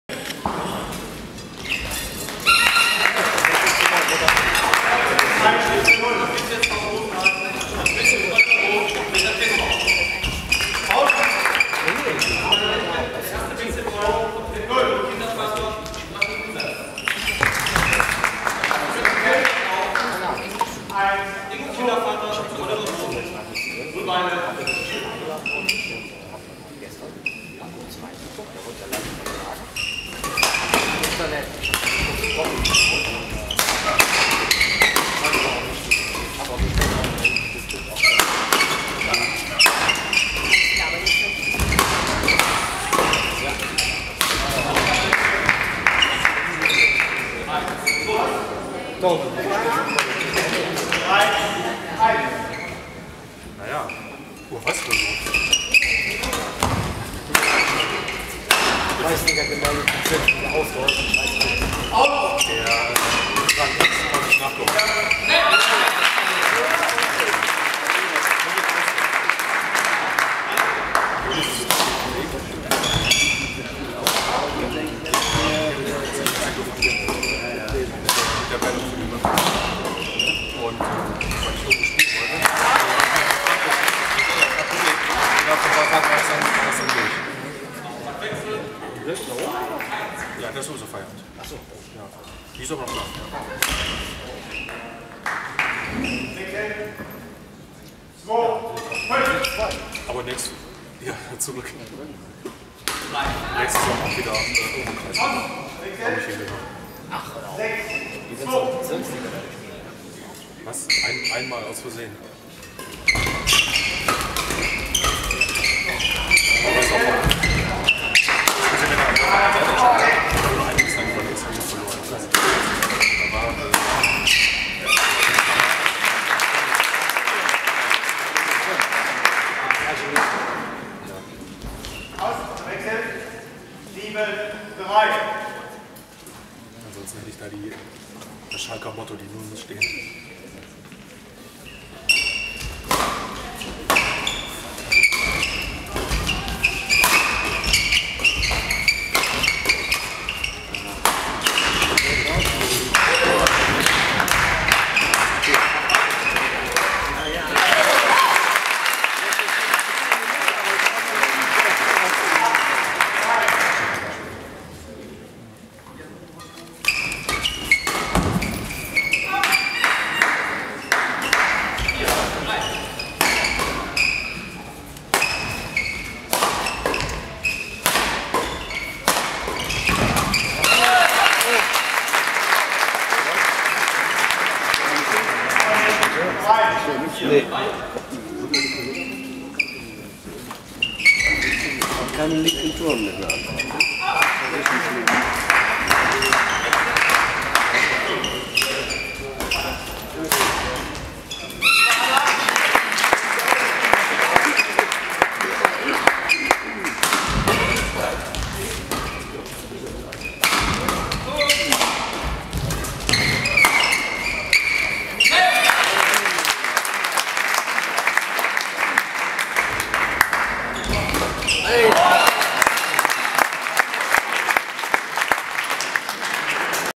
Badminton Bundesliga Michels & Pohl vs. van Dooremalen & Bellenberg Satz 1 Teil 1
Erwin Kranz Halle Bonn Beuel